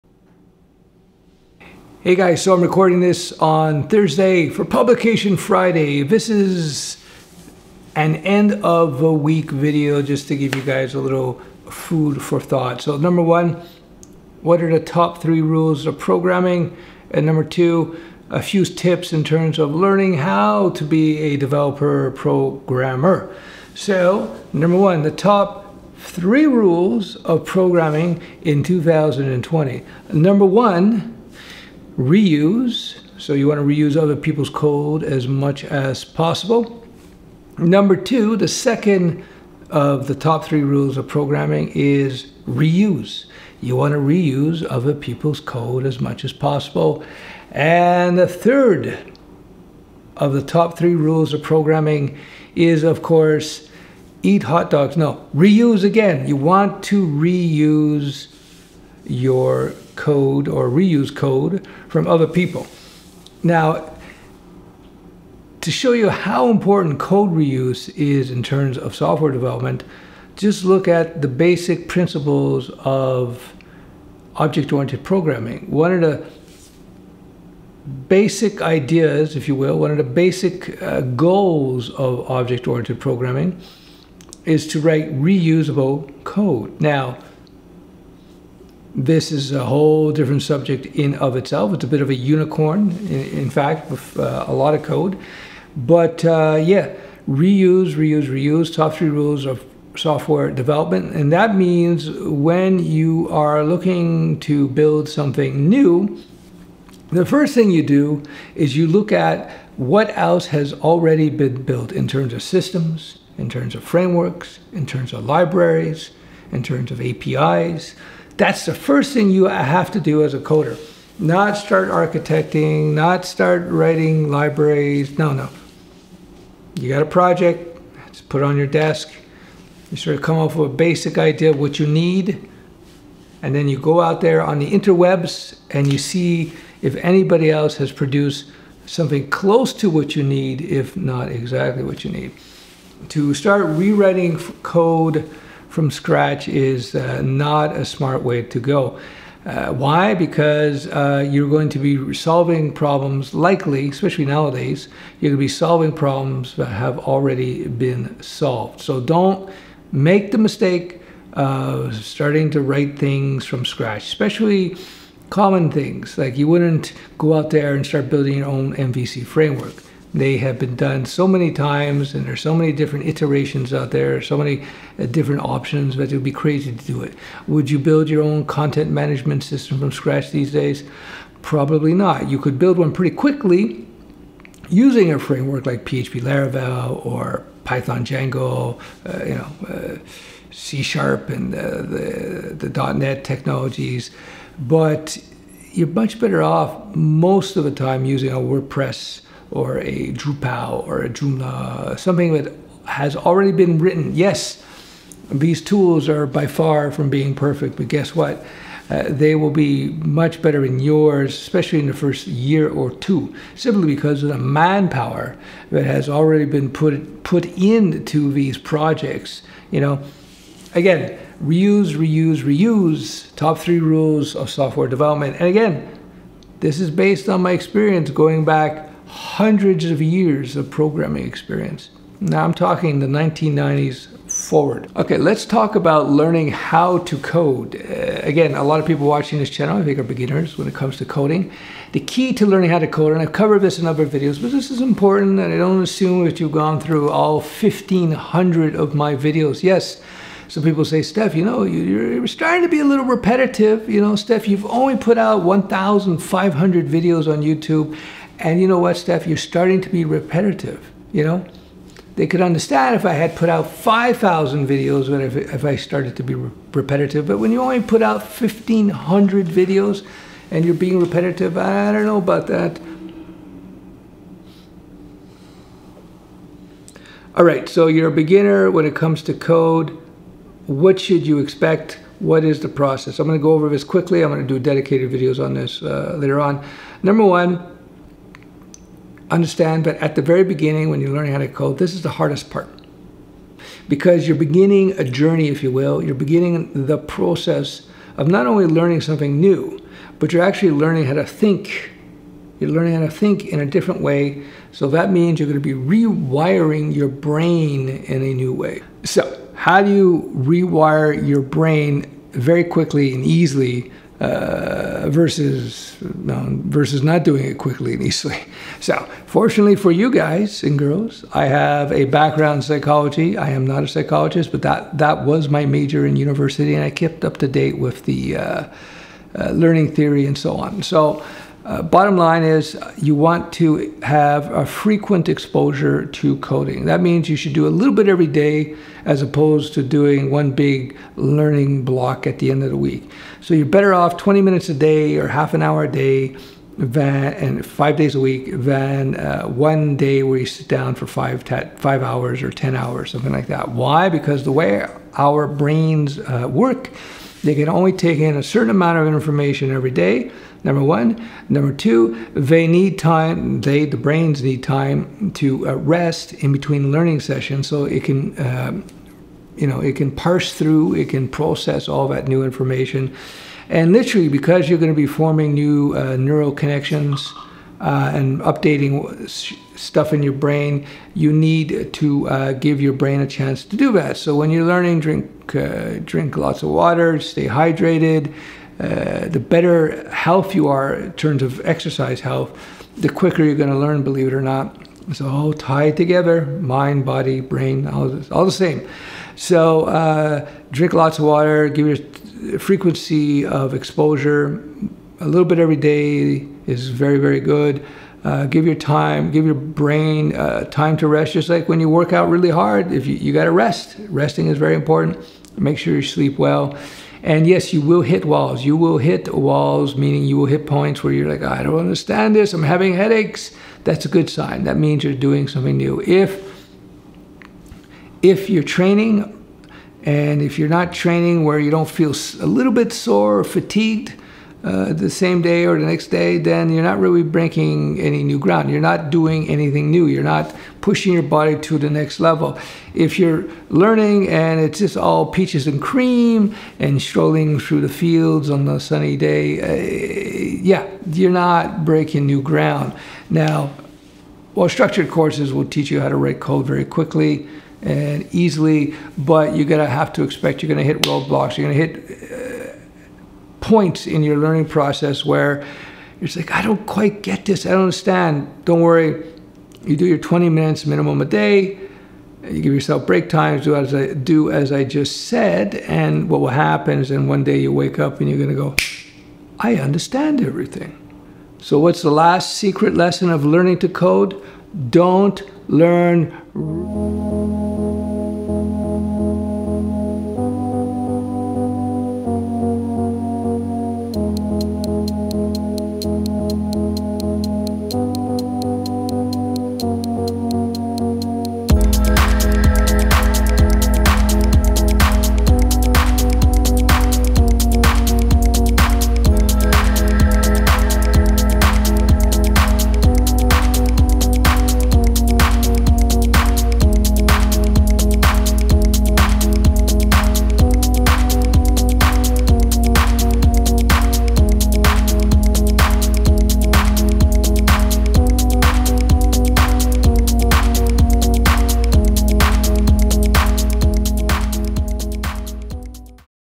Top 3 Programming Myths and Learning Tips for 2020
Your friendly neighborhood 169yr old developer is laying down top tips that will help you with all your problems.

Sit back, have a coffee and relax ... let the ancient nerd-wisdom permeate your brain cells like craft coffee.

Sign up to my Need2Nerd Newsletter for exclusive nerd content that everyone loves

Thanks!
Stef